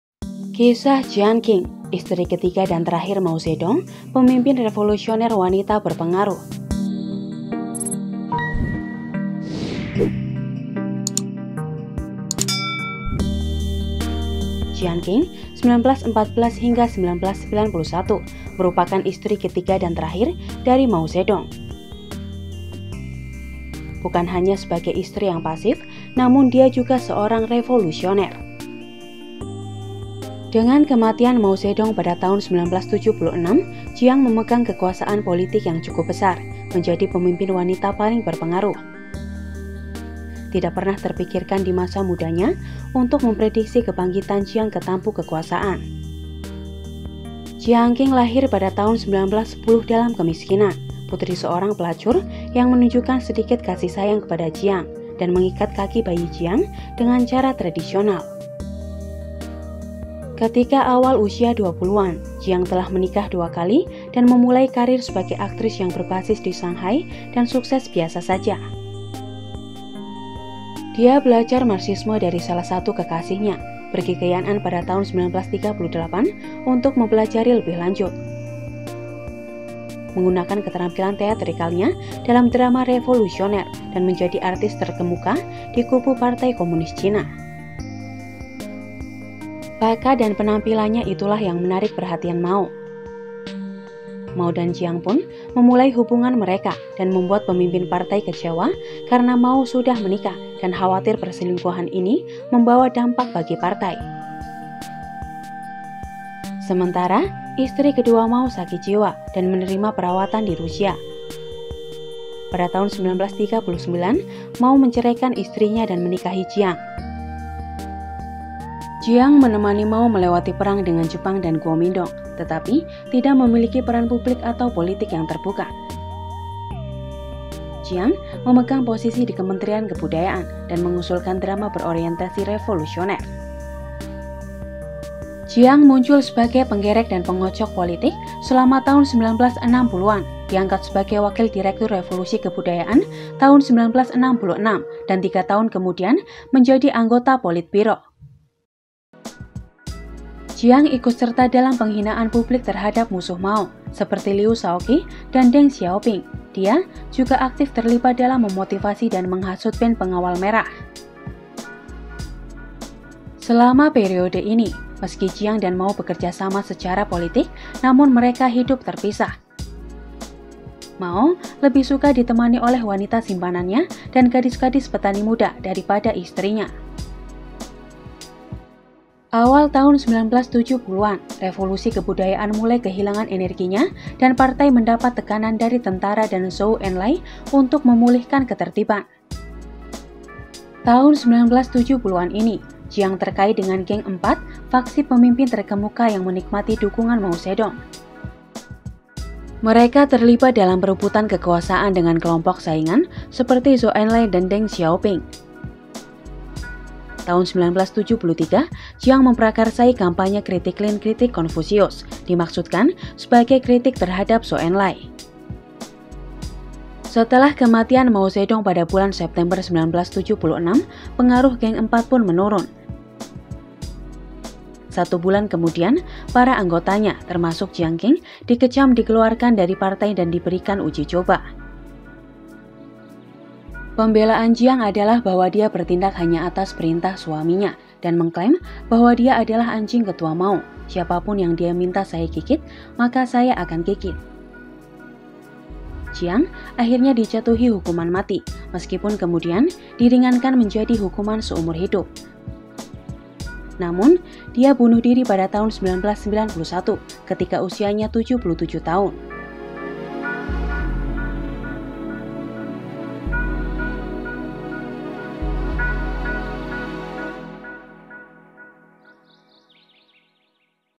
Inilah Kisah dari Jiang Qing, Istri Ketiga dan Terakhir Mao Zedong
Jiang Qing adalah istri ketiga dan terakhir Mao Zedong. Dengan kematian Mao, maka Jiang memegang kekuasaan politik yang cukup besar.